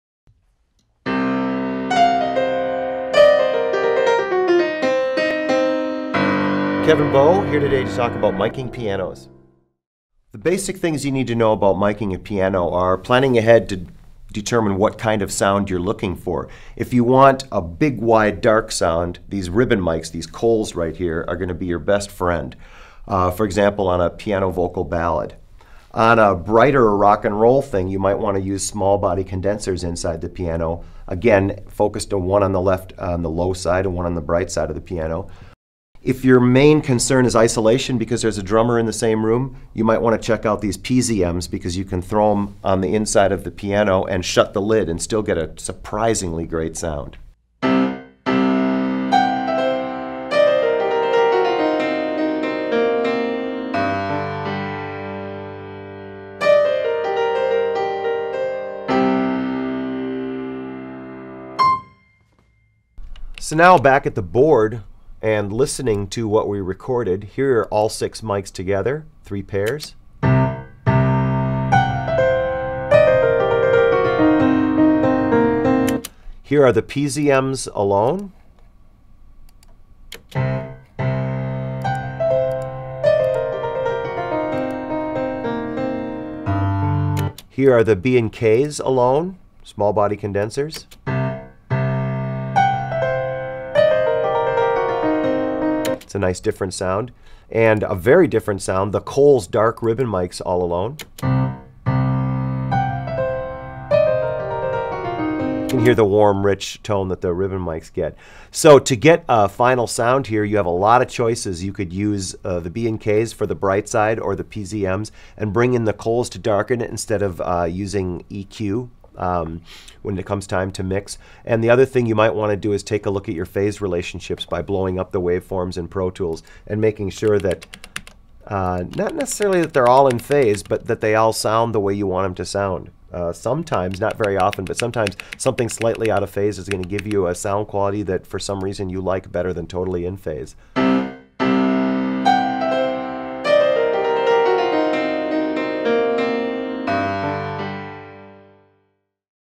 IPR | How to Mic a Piano: Recording Piano Techniques
Wondering how to record a piano? Kevin Bowe of IPR demonstrates how to mic a piano and the different factors to consider such as microphone placement and different piano microphones.

Learn these techniques and more in our audio production & engineering program